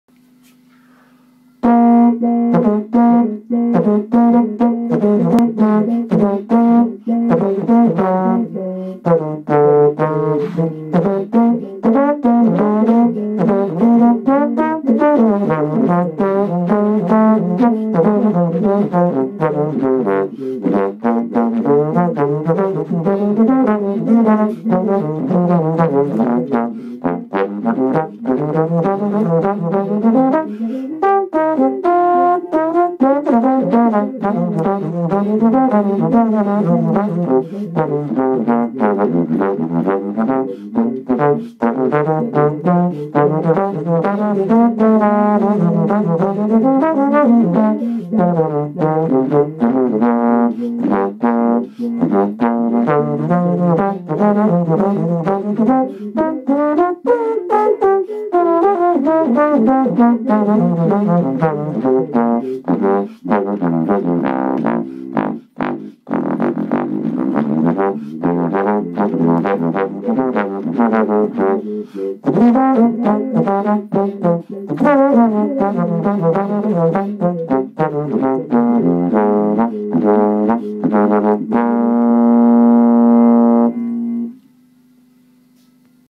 Morning in the basement
Playing around with a couple of pedals whilst warming up this morning...